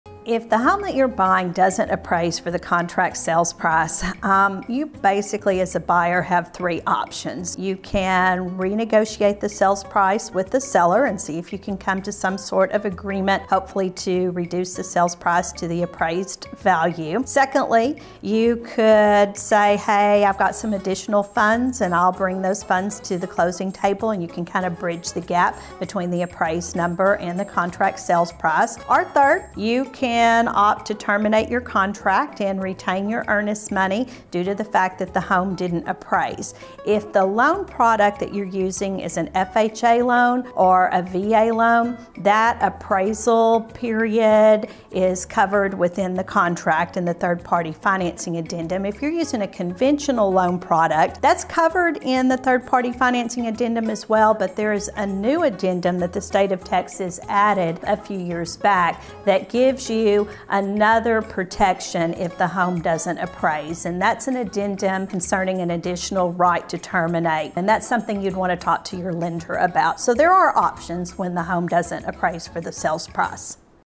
Minute with Melissa - Appraisal Challenges and Solutions
Appraisal challenges? Melissa tackles this tough topic in our latest video! 🏡💼💰